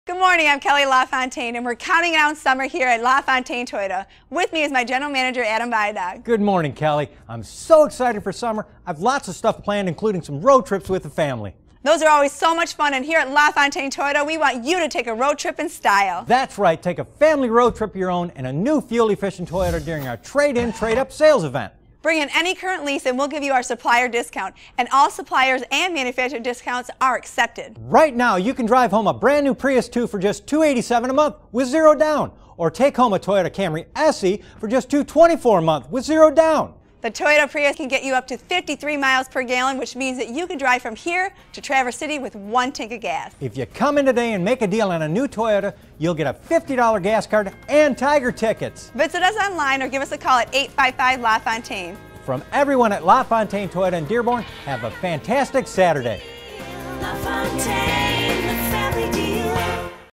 LaFontaine Toyota - Trade In - Trade Up Sales Event - Dearborn, MI
Kelley LaFontaine & Adam Bidock of the LaFontaine Automotive Group are here today with the LaFontaine Toyota Trade In - Trade Up Sales Event Specials. Check out the video for all the details.